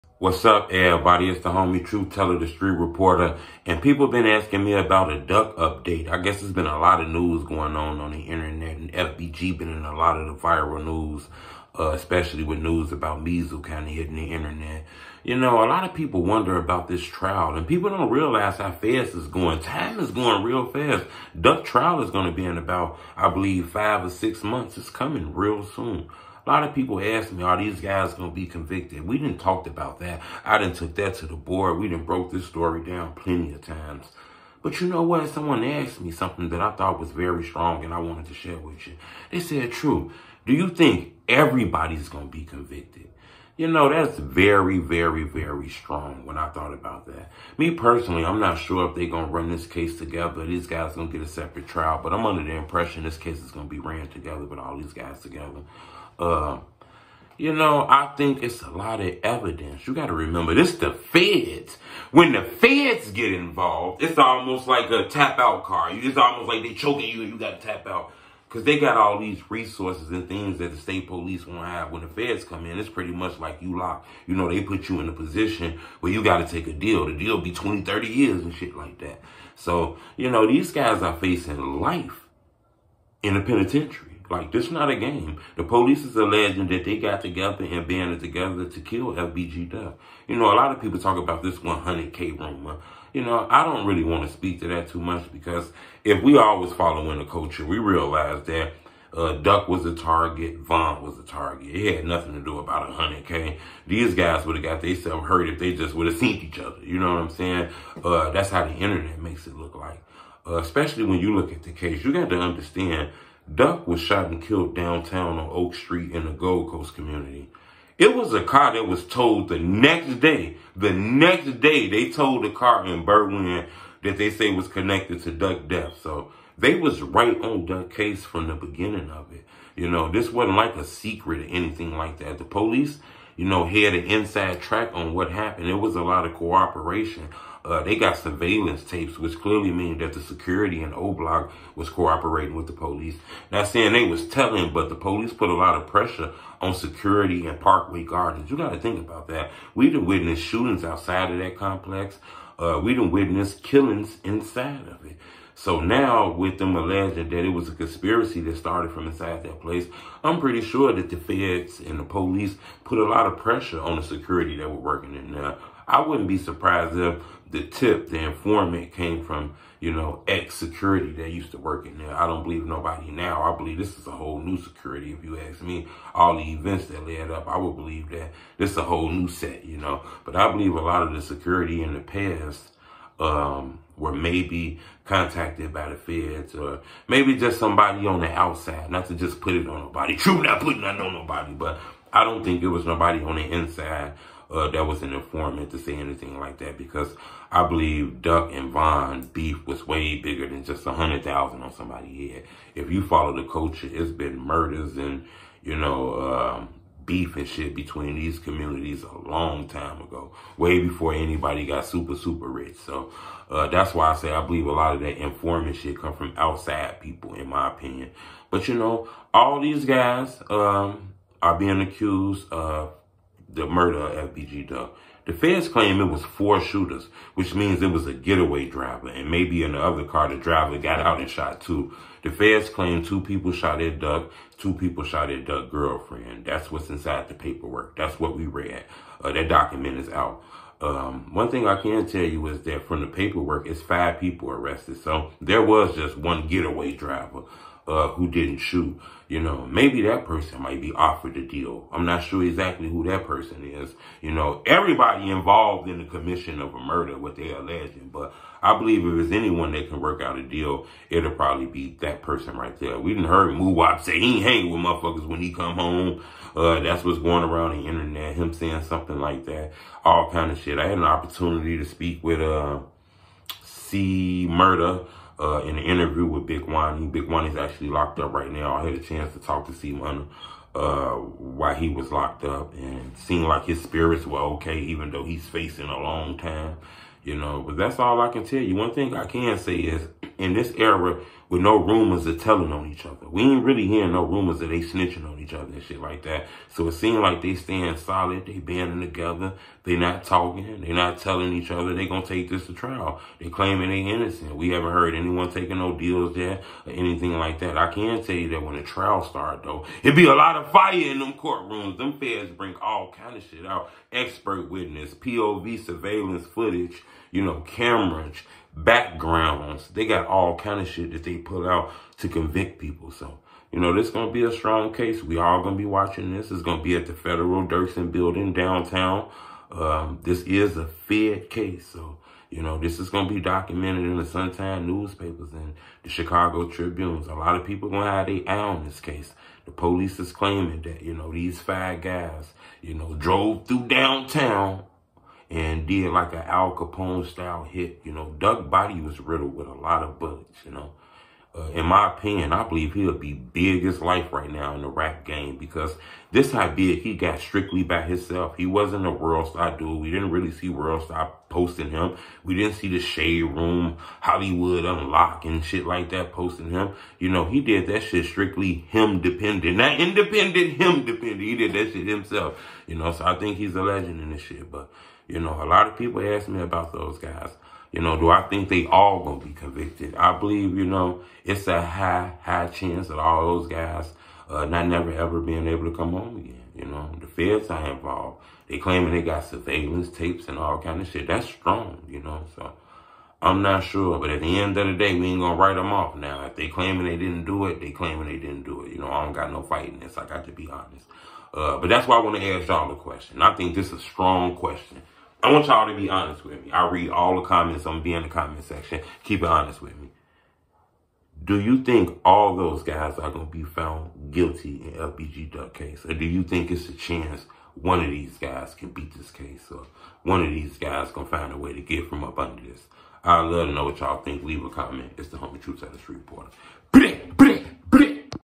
The Reason Why The FEDS Put Pressure On Past Oblock Sercurity Guards After FBGDuck Death (Allegedly)
FEDS Put Pressure On Past Oblock Sercurity After FBGDuck Death